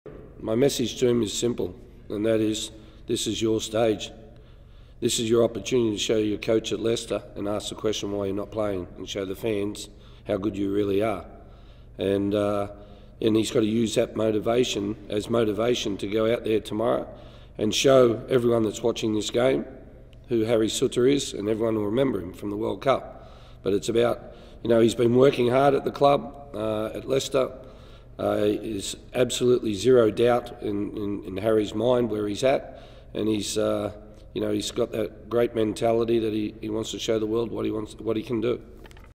Arnold - 'This is Souttar's stage to remind everyone how good he is' | Mexico v Socceroos Preview
Despite being frozen out of the playing squad at Leicester City, Socceroos head coach Graham Arnold has said he has no fear about starting the big defender against Mexico.